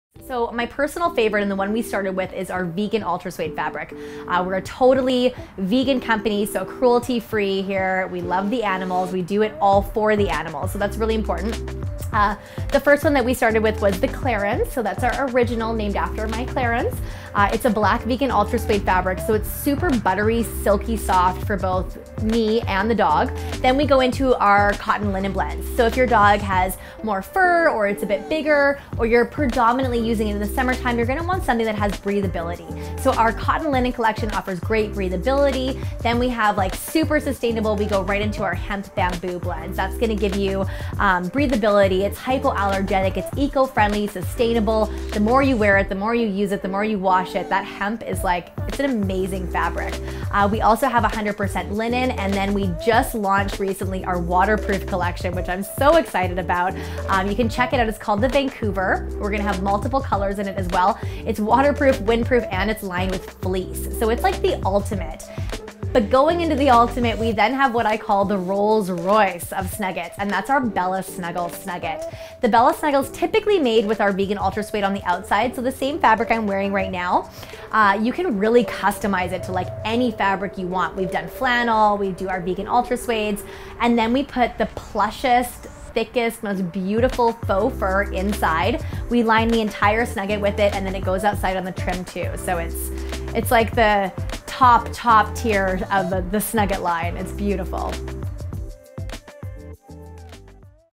Let's talk about our fabric!
We are OBSESSED with fabric! We've spent so much time looking, touching and sourcing the softest most beautiful, durable, comfortable and sustainble fabric for you and your pup! We have a variety of sustainable fabrics ranging from our lightest weight hemp SNUGGIT (The Chino) into our  cotton linen blends to our silky soft vegan ultra suede SNUGGITS and  then the most snuggly of them all The BELLA SNUGGLE fabric where we use the highest quality faux fur on the inside and the vegan ultra suede on the outside.
All our fabrics are vegan, sustainable and MACHINE WASHABLE!
Please let us know your favourite fabric in the comments below!